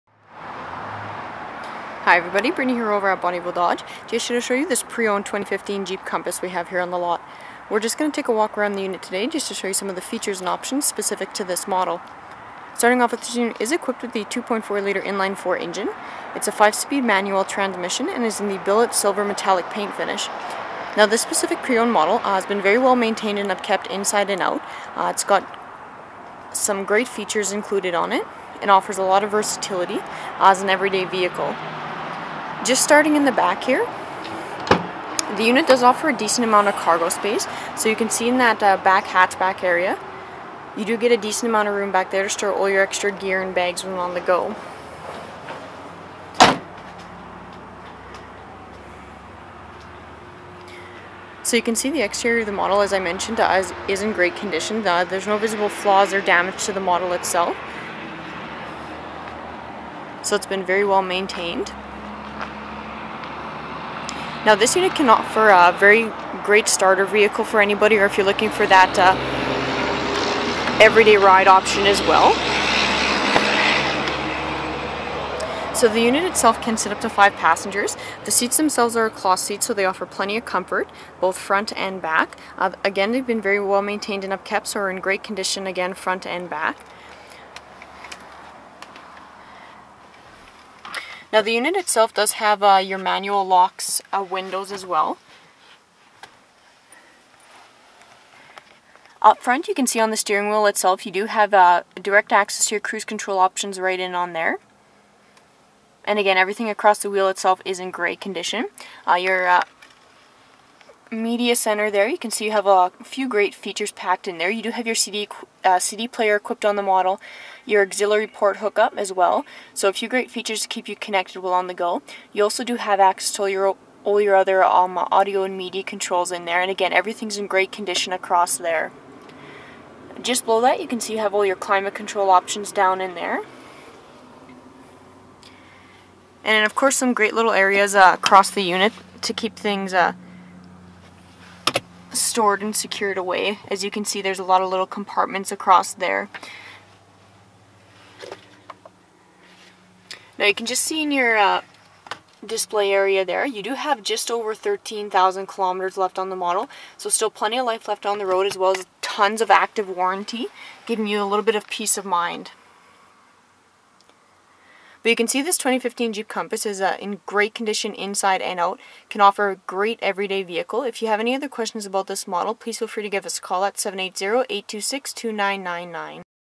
2015 Jeep Compass Sport/North
Like new condition and under 14,000kms are just two of the reasons this 2015 Jeep Compass would make a perfect option for your next vehicle. Every feature is in almost the same condition as the first time it was driven away from the dealership. You could be driving away in a pre-owned vehicle at a pre-owned price with like new conditions from front to back. To top it off thanks to the incredibly low km's this unit has you will also be driving away with the peace of mind of having active warranty on your purchase.